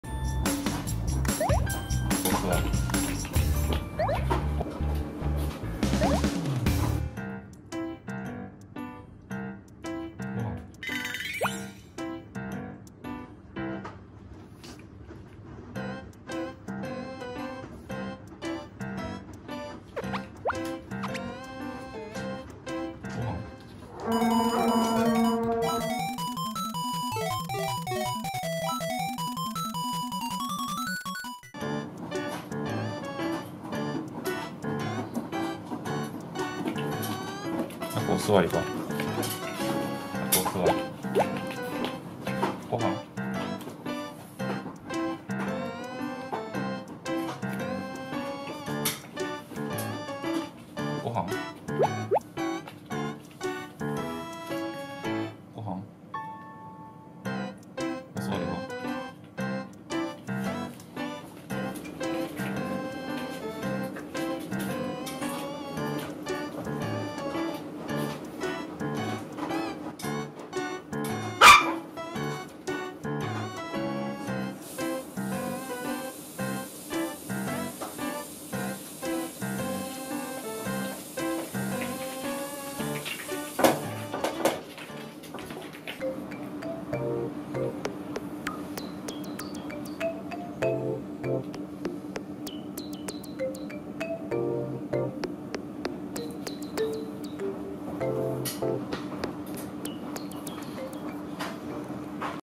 ポメラニアンのご飯前の様子【70日目】
ご飯と言うと何をしてても頭がそれだけになってしまうラクです。今日も元気にご飯食べてました。